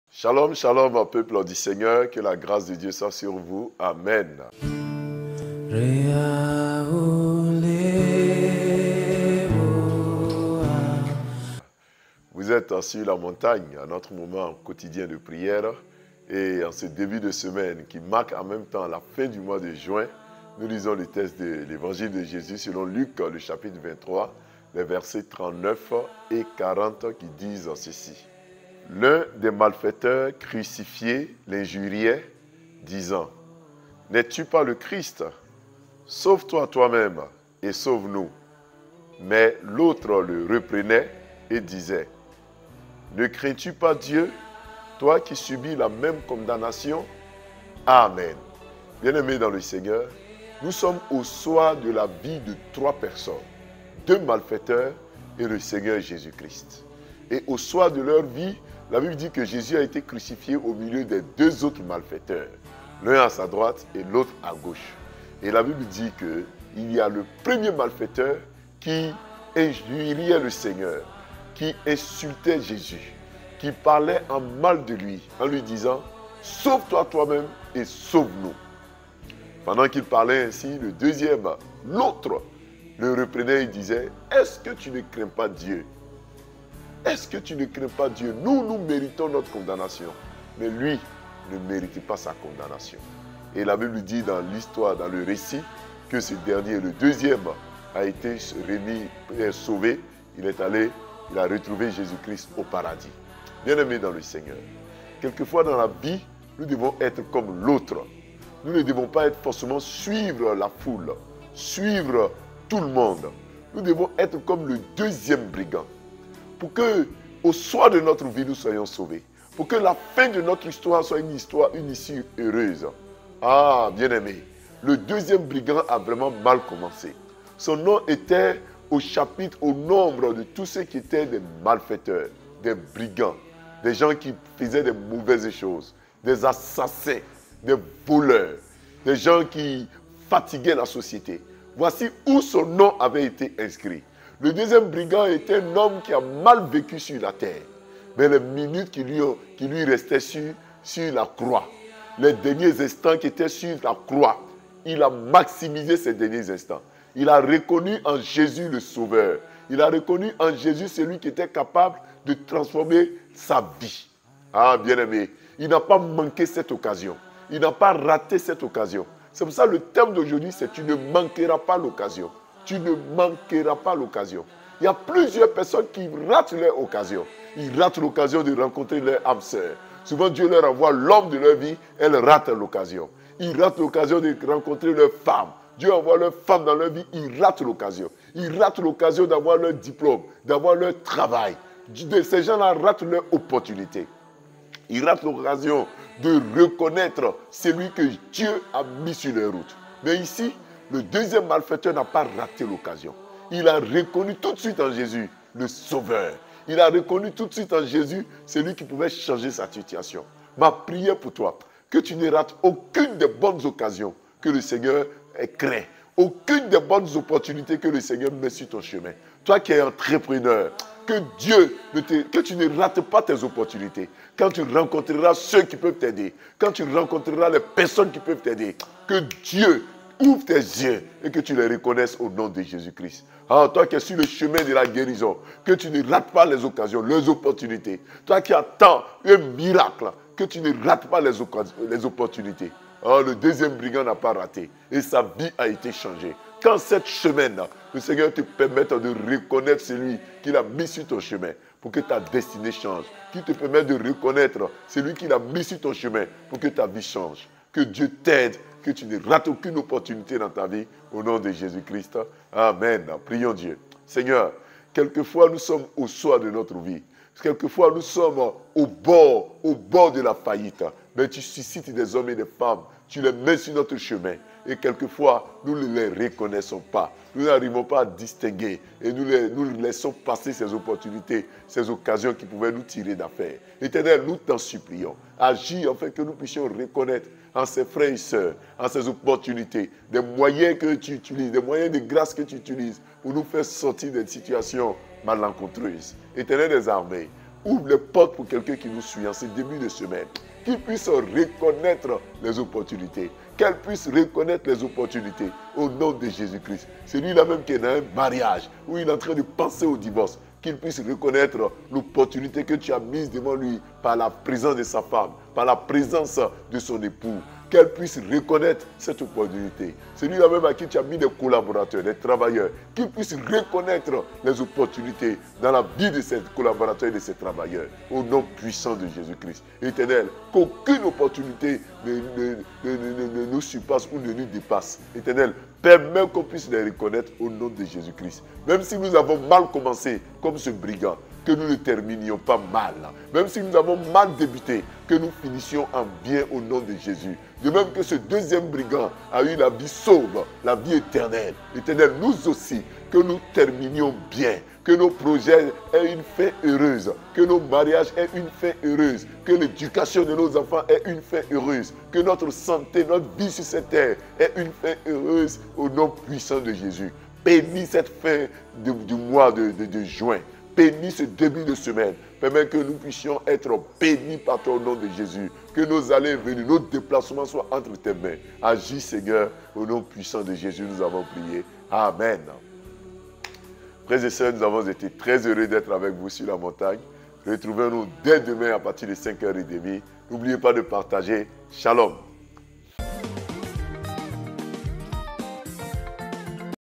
𝗦𝗨𝗥 𝗟𝗔 𝗠𝗢𝗡𝗧𝗔𝗚𝗡𝗘 Thème : Tu ne manqueras pas cette occasion. Luc 23:39-40 Pasteur Ehua De Monga
𝗦𝗨𝗥 𝗟𝗔 𝗠𝗢𝗡𝗧𝗔𝗚𝗡𝗘
Thème : Tu ne manqueras pas cette occasion. Luc 23:39-40
Pasteur Ehua De Monga

Prière pour confier sa journée à Dieu.